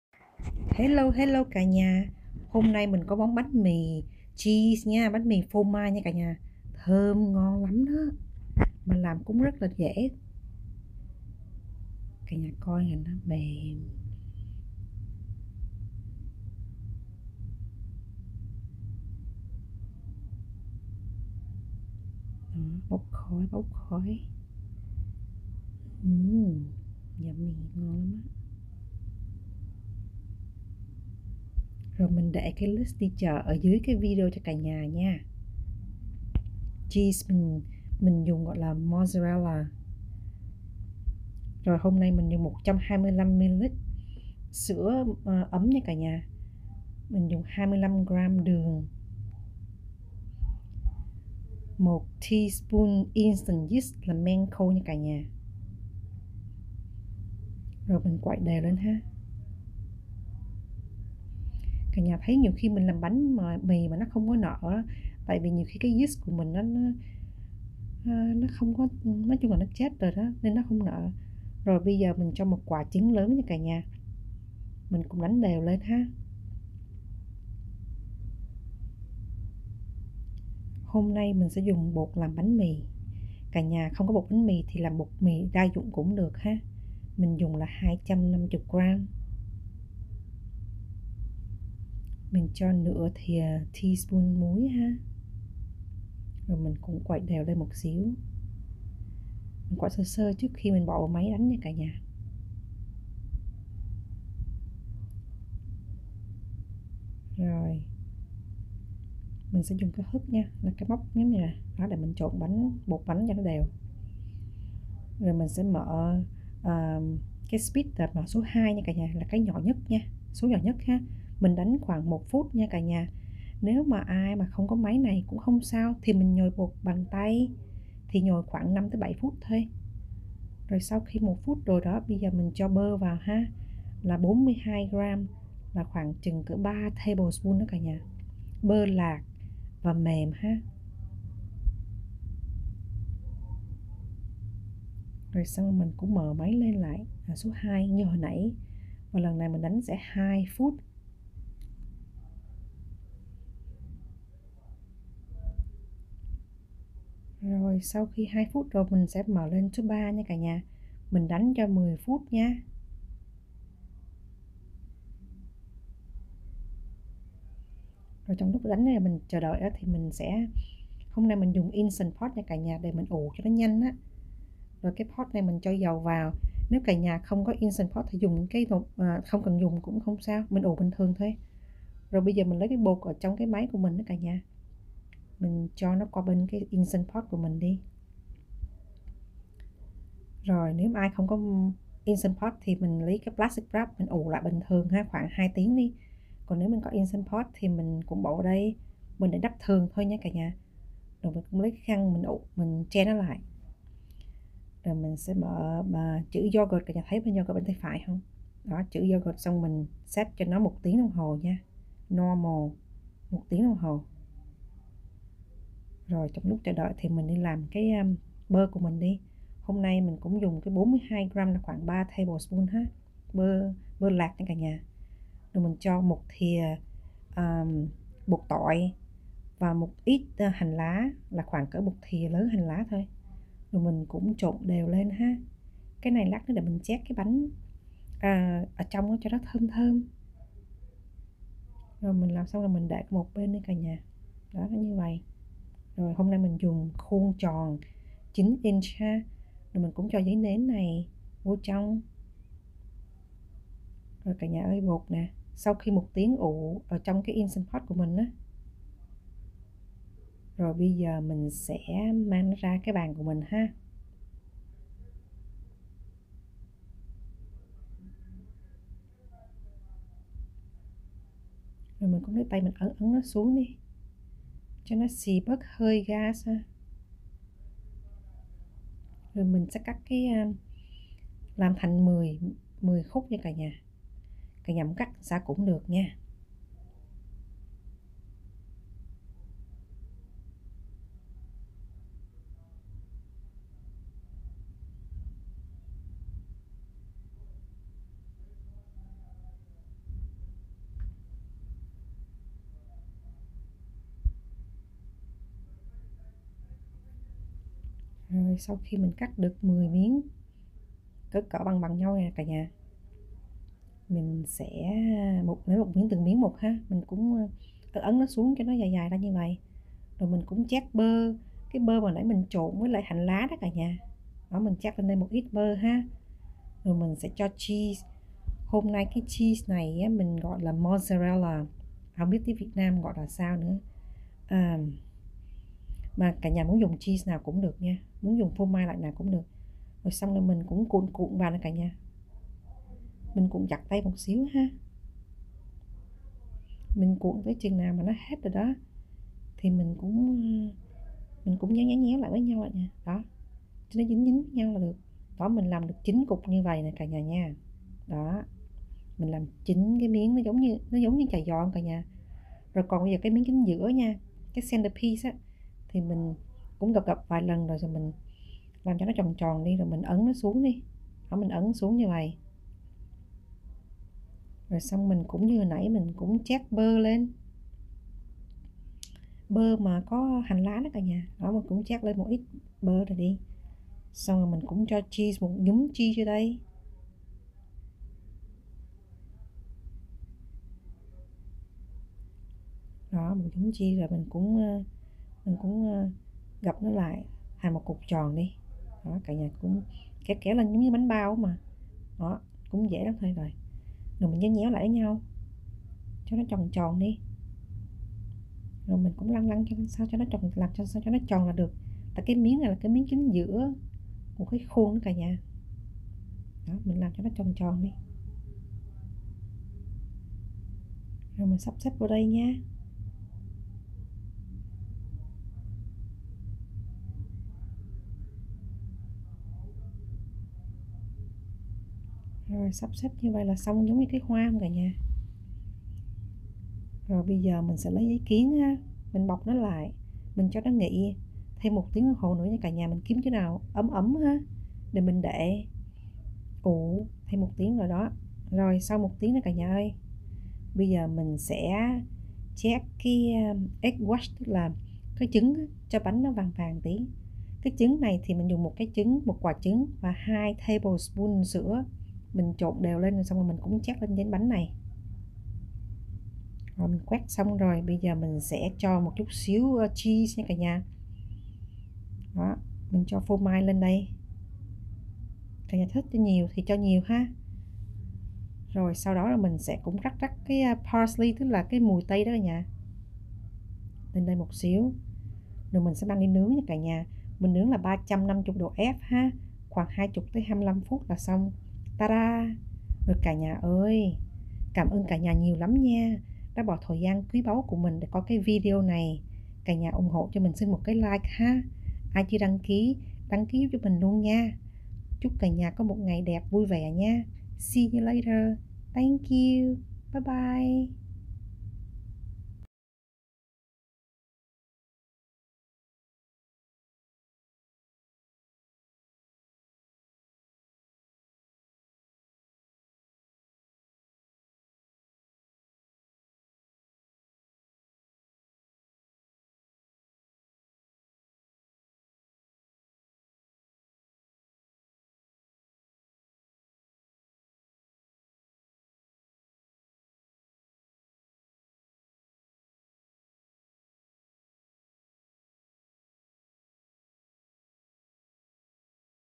Bánh Mì Phô Mai, bơ, hành tỏi thơm mềm và Xốp công thức tiếng Việt - Mozzarella Cheese Bread
Ingredients :

125ml warm milk (1/2 cup)
25g sugar (2 Tbsp)
1 tsp instant yeast
1 large egg
250g bread flour OR all-purpose flour (2 cups)
1/2 tsp salt
42g soft unsalted butter (3 Tbsp)

9 inch round pan

For filling and topping:
120g-150g shredded mozzarella cheese. (1 - 1 & 1/4 cup)

For filling:
42g (3 Tbsp) soft butter + 1 Tbsp chopped scallions + 1 tsp garlic powder

** Bread flour
** All purpose flour
** Instant yeast
** Stand mixer
** Non-slip silicone pastry mat
** Instant Pot